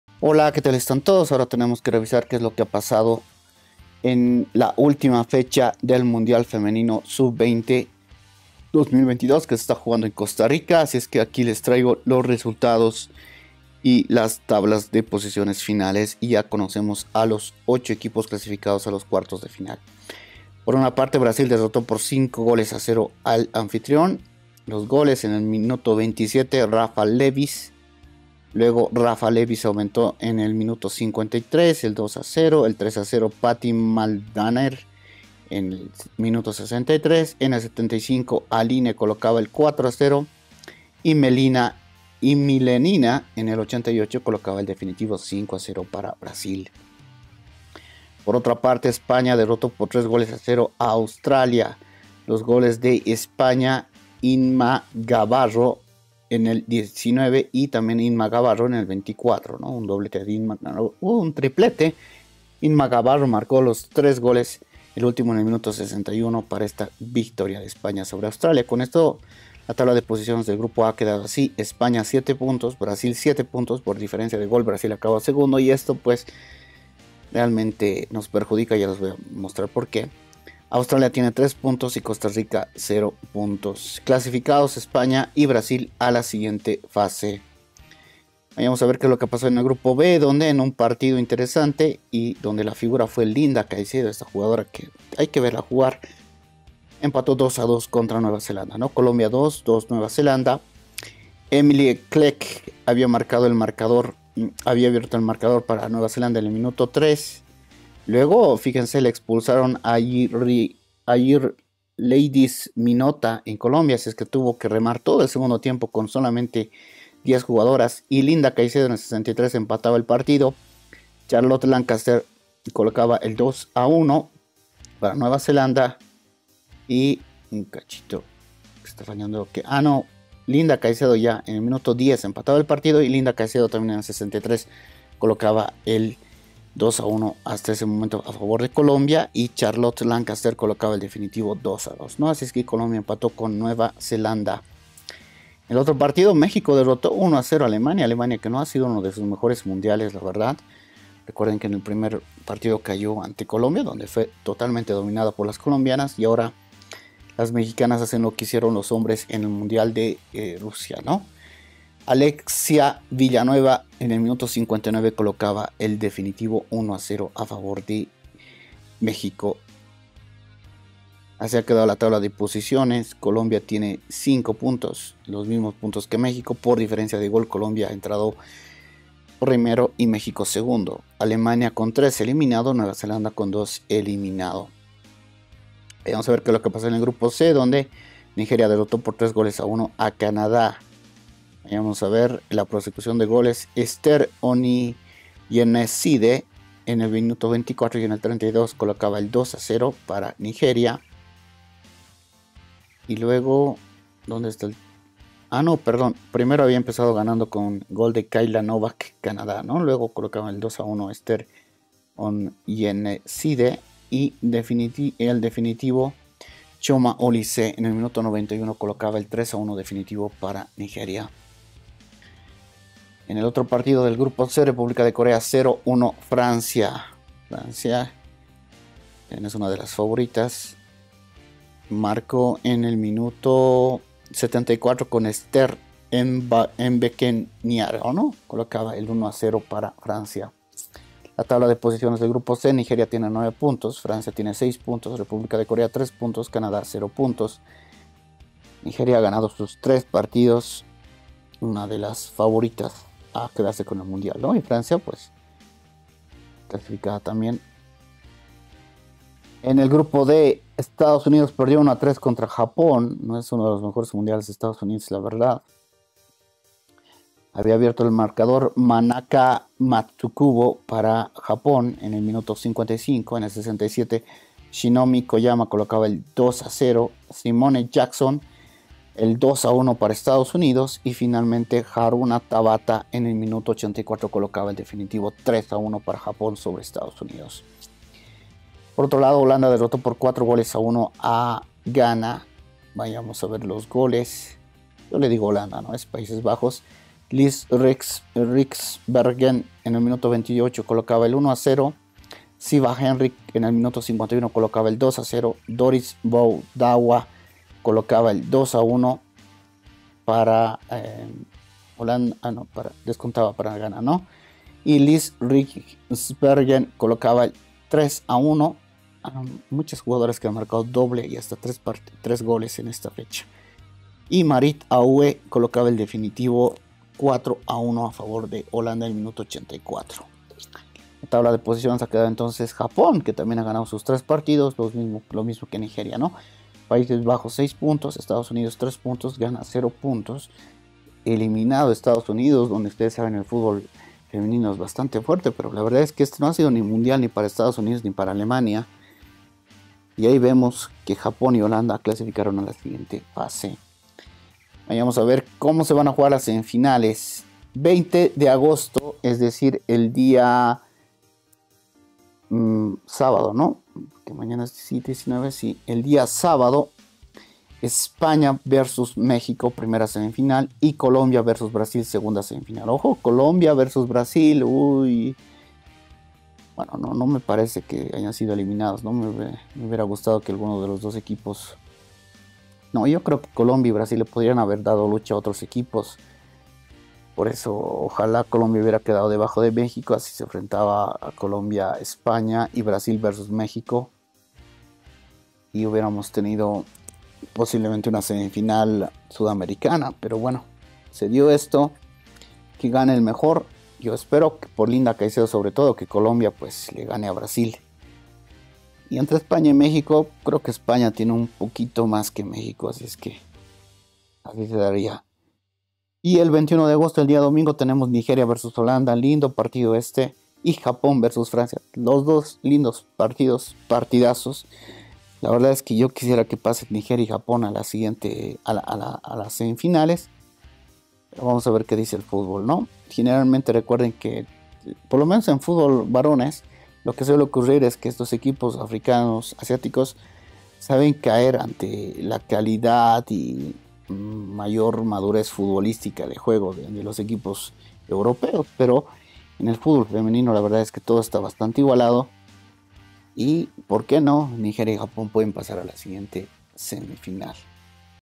Mundial Femenina Sub 20 de futbol 2022, Fecha 3: Resultados, Posiciones y Cuartos de final
Ahora revisamos los resultados de la tercera fecha del Mundial Femenina Sub 20 de futbol 2022, ya tenemos a los 8 clasificados a la siguiente instancia